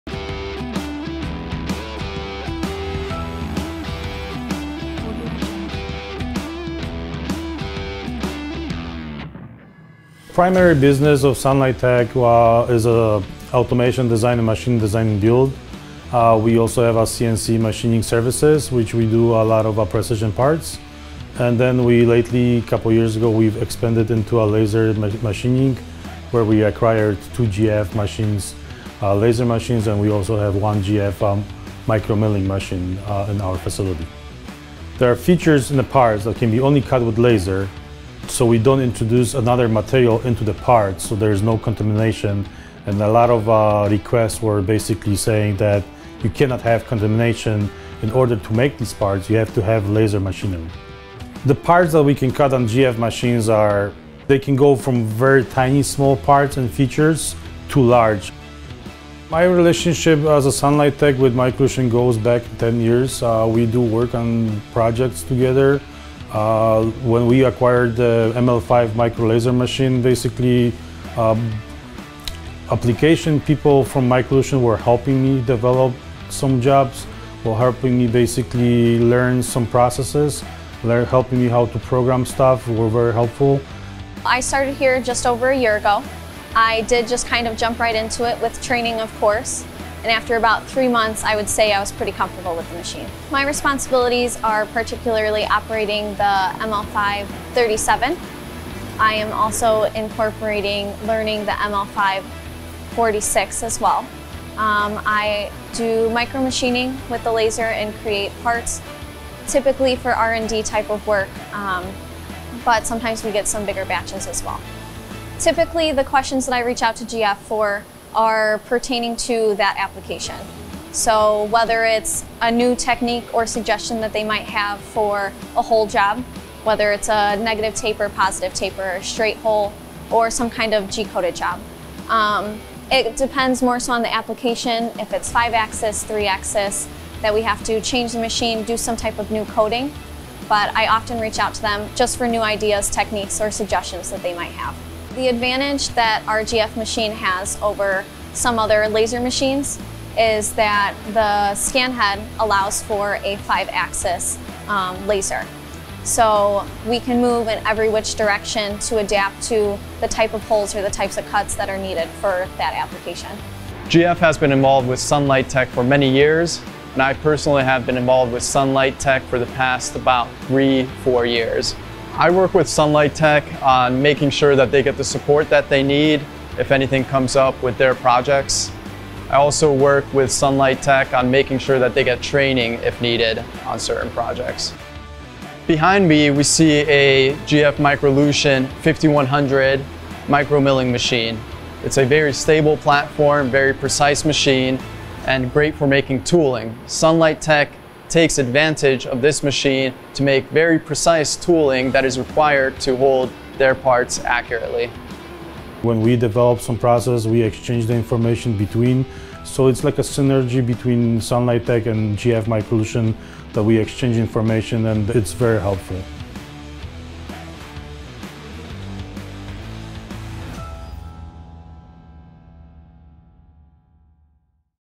Sunlight-Tech Case Study
Sunlight-Tech Inc. is a femtosecond laser contract manufacturer company & an automation design and machine design build manufacturer based in Mokena, IL.  Utilizing the GF Microlution ML-5 micro laser machine and the 5100 Micro-Milling Center, Sunlight-Tech manufactures precise and intricate parts to ensure the satisfaction of their customers.  Take a look at the strong partnership that GF Microlution has established with Sunlight-Tech Inc.